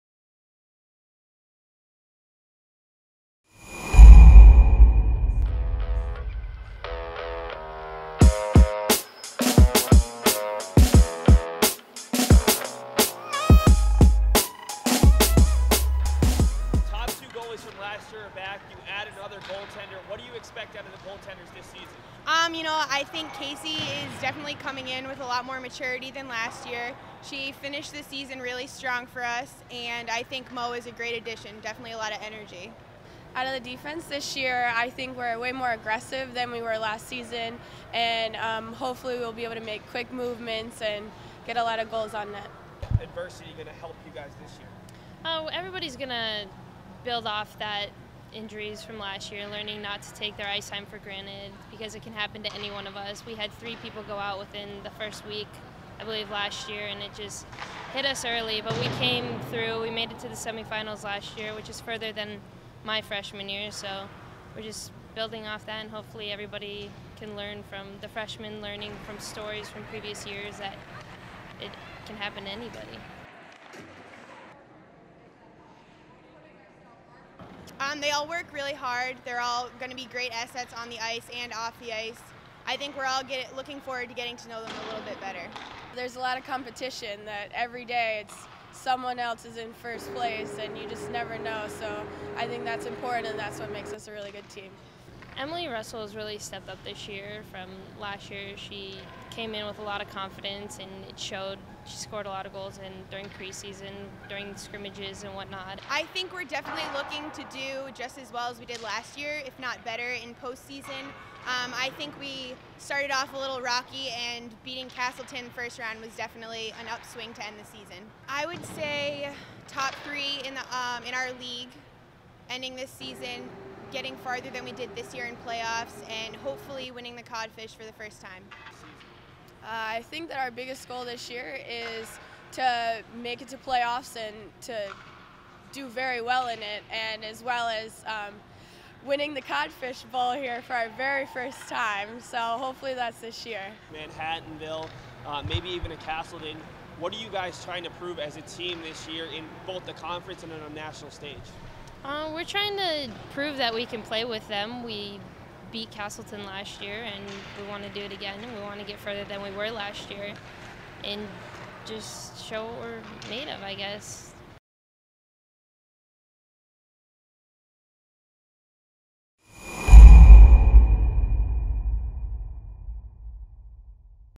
2013-14 UMass Boston Women's Hockey Preview
Juniors Kailyn Burke, Lauren Tansey and Alexandra Karlis talk about the upcoming 2013-14 season which begins Friday November 1st. The three juniors discuss the Beacons positive outlook and high expectations for the 2013-14 season. (Video done by Andy Bukuras '16)